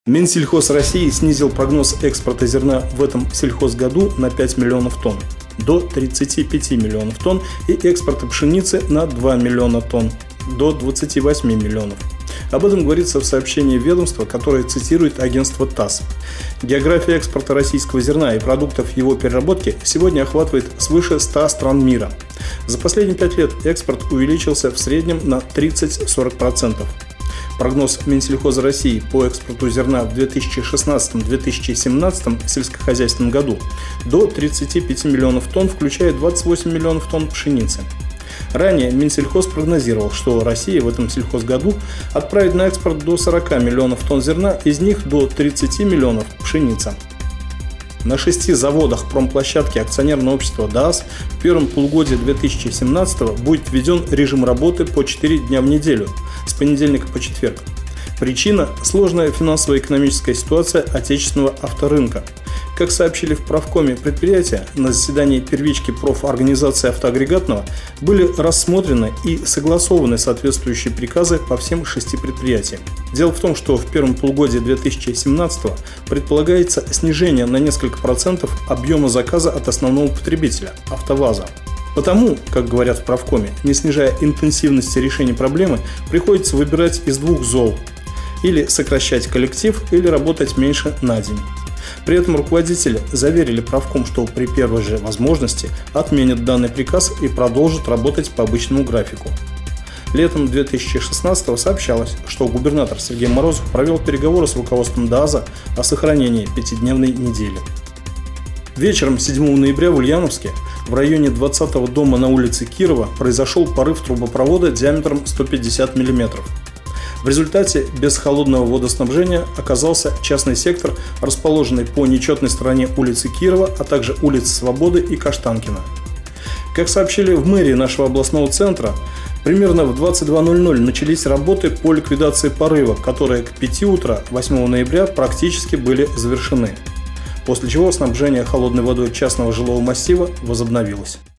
ЭКСПОРТ ЗЕРНА, ЧЕТЫРЁХДНЕВКА НА ДААЗЕ, ХОЛОДНЫЙ ПРОРЫВ
Блиц-новости России, Ульяновска и Ульяновской области